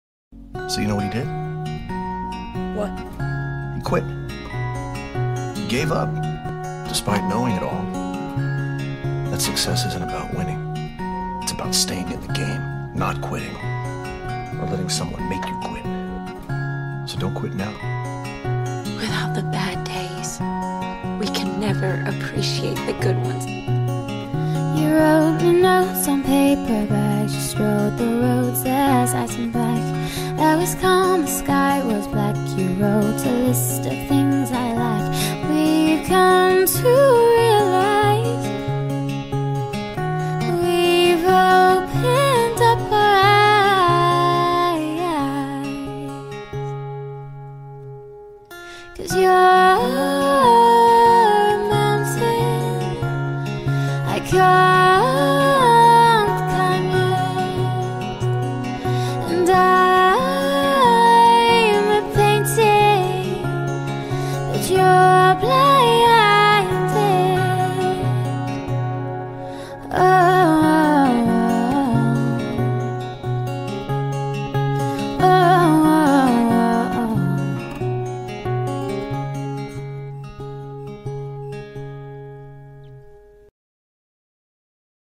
Success isn't about winning -_-_-_-
Okay so I was in the mood to edit a video today :) All new footage (except the parts showing Holly!)

I'm doing okay health wise, I'm currently waiting to have an echocardiogram done and see a cardiologist... Such fun ;P

Enjoy! :D I will SUB 4 SUB

Overlay & footage by me :)